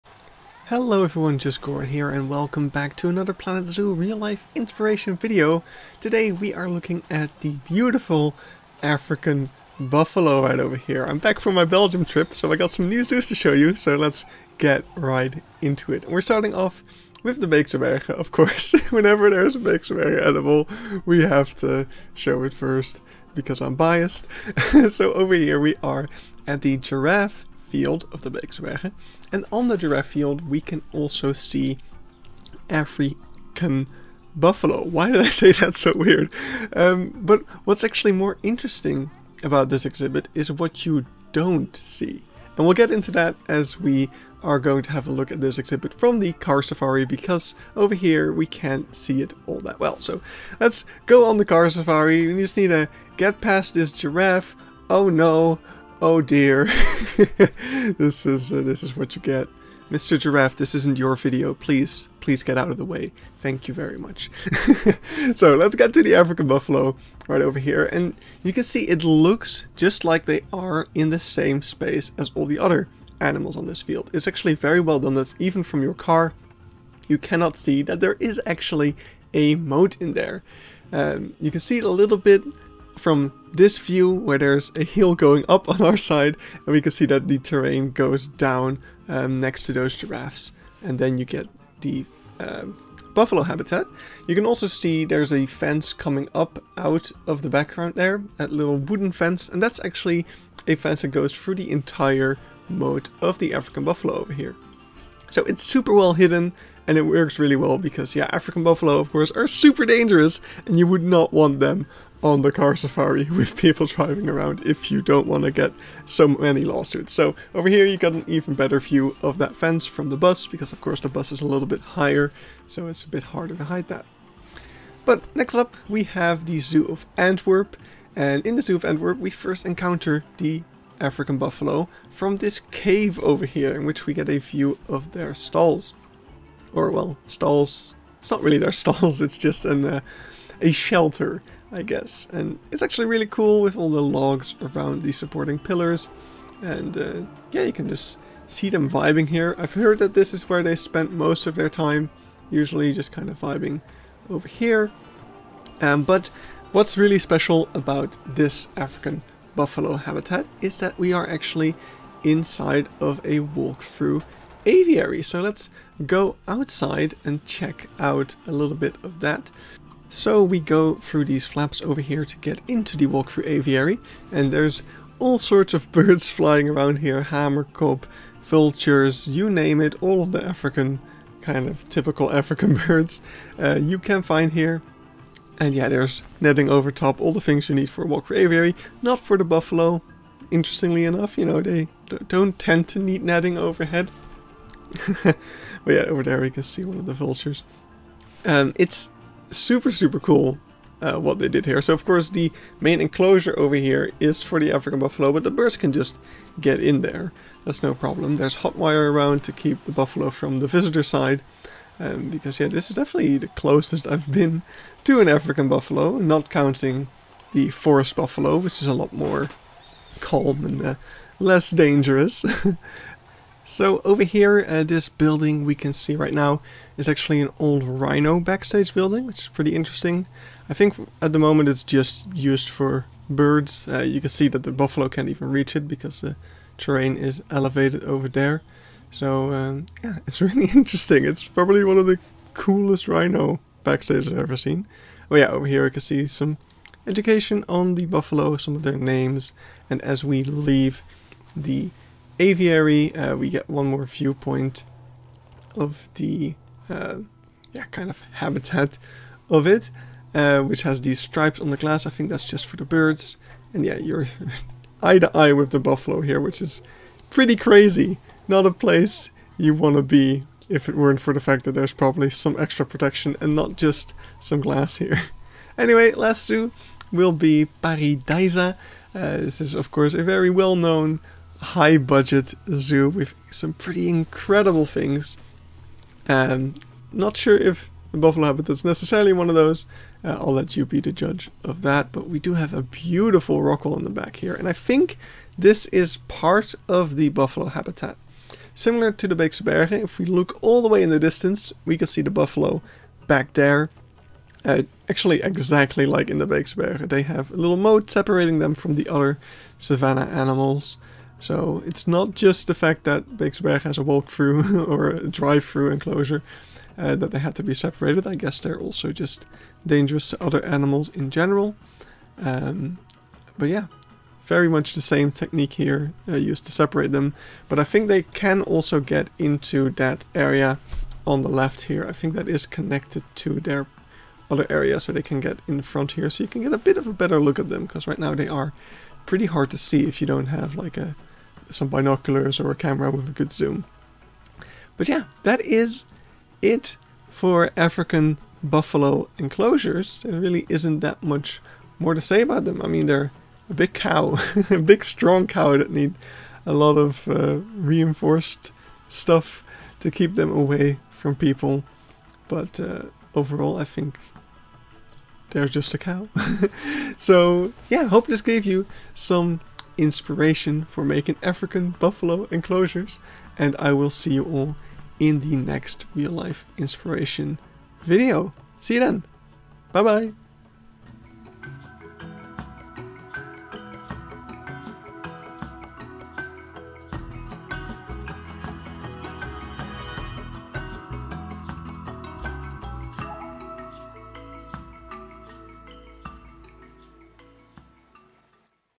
3 Real African Buffalo Habitats! (Planet Zoo Inspiration)
I'm back from my Belgium trip! Now I have few more zoos to show you all! Today we're looking at the African Buffalo!

00:00 Intro
00:16 Beekse Bergen
02:03 Antwerp
04:33 Pairi Daiza

Twitter me @justGoron!